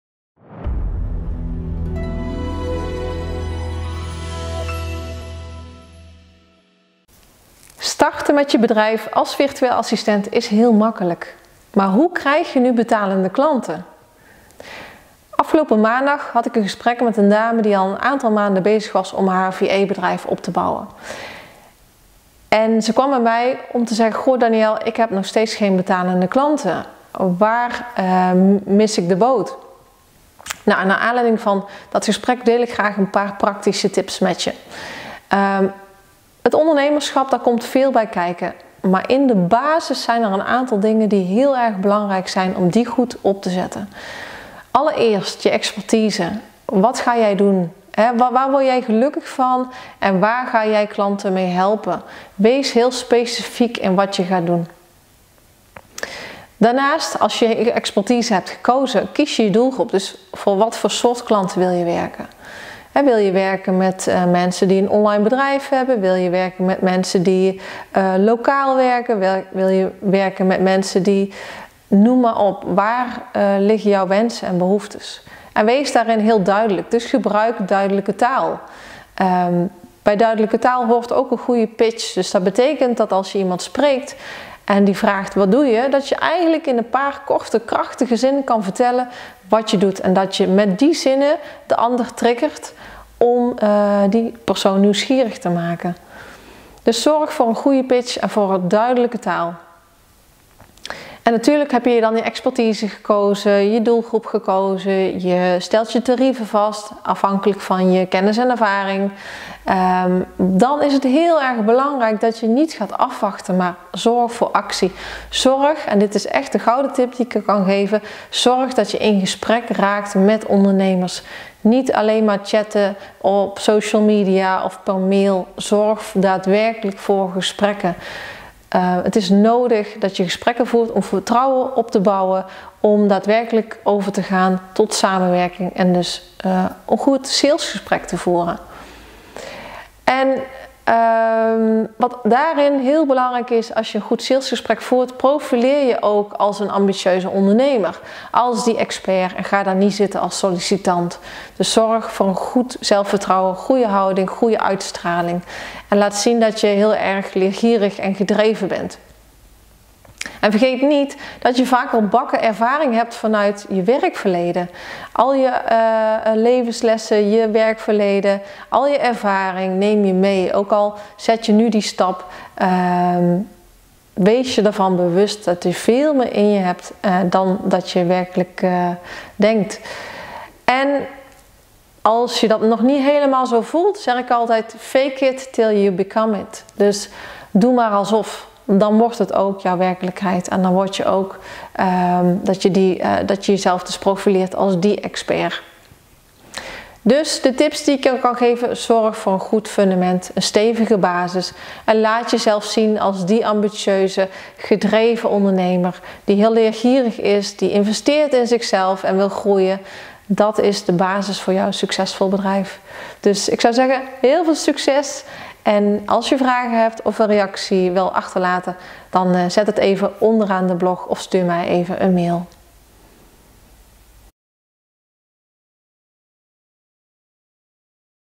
Virtual Assistant? Dit is je basis voor betalende klanten
Maandagochtend had ik een gesprek met een VA die inmiddels een paar maanden bezig was. Ze was nog steeds aan het zoeken naar haar eerst betalende klant. Inmiddels had ze de basis van haar bedrijf staan. Haar website was ook in de lucht. Ook was ze bezig om zichtbaar te worden in de markt. Ze wilde immers betalende klanten. Want een gezin runnen, daar is natuurlijk inkomen voor nodig. Maar helaas, bij haar bleven de klanten uit.

Wat maakt nou dat ze nog geen klanten had? Ondernemen lijkt soms zo simpel. En ja, feitelijk gezien is het dat ook. Je gaat naar de Kamer van Koophandel of vraagt je ondernemersnummer aan en je hebt een bedrijf.

Maar heb je dan ook echt een bedrijf? Als je het mij vraagt. Ja, je hebt een bedrijf, maar een bedrijf is pas een écht bedrijf als het ook succes heeft want anders is het gedoemd om te mislukken. Dan stop je ermee.

Wat had deze VA nu kunnen doen om meer ‘in the picture te komen’ bij potentiële klanten?
Daar is niet 123 een simpel antwoord op te geven. Het runnen van een succesvol bedrijf heeft meer voeten in de aarde.

Het is in ieder geval belangrijk om de basis goed op te bouwen.

Dat doe je door heel specifiek je expertise te kiezen. Waar word jij blij van? En waar heeft jouw klant behoefte aan? Wees helder in je aanbod. Gebruik duidelijke taal zodat je potentiële klant weet wat er bij jou te kopen is.

En voor wie wil jij werken? Wat voor type ondernemers? Waar ligt de vraag in de markt? Ga op onderzoek uit en zorg dat je weet welke ondernemer bij jou past.

Als je dit weet kun je een goede pitch bedenken zodat jij jouw woord gaat verspreiden. Zorg dat je in een paar zinnen zegt wat je doet en voor wie. Ook hier gebruik je duidelijke taal. En blijf dan niet afwachten of alleen maar mailen. De gouden tip die ik je hiervoor geef is: zorg dat je in gesprek raakt met potentiële klanten. Als je betalende klanten wilt, dan is het belangrijk dat je gesprekken voert. Zorg voor interACTIE!

En dan begint een spannend stuk voor de meeste. Want wij vrouwen zijn vaak bescheiden. We willen niet zomaar ‘hoog van de toren blazen’. Ondanks dat we soms BAKKEN ERVARING hebben!

Wow daar schrik ik toch wel eens van. Hoe terughoudend sommige zijn met zo veel ervaring. En dat vind ik doodzonde. Want als je jezelf presenteert als een startende vA-player (virtuele A-player) dan denken de meeste dat ze zich als een startende ondernemer moeten opstellen.

Wrong! Fake it till you become it! Doe maar alsof je die succesvolle virtual assistant bent. Dan kijken potentiële klanten heel anders tegen je op. Dus op dit vlak valt nog ongelofelijk veel winst te behalen.

Bouw dus je basis stevig op, gebruik duidelijke taal en laat jezelf zien als een ambitieuze gedreven ondernemer.

Op jouw succesvol bedrijf!

Enthousiaste, ondernemende groet,

Daniëlle Leuvering

VA Business Academy in Nederland en België

Voor ambitieuze moeders die gaan voor een winstgevend bedrijf in eigen tijd als virtueel assistent.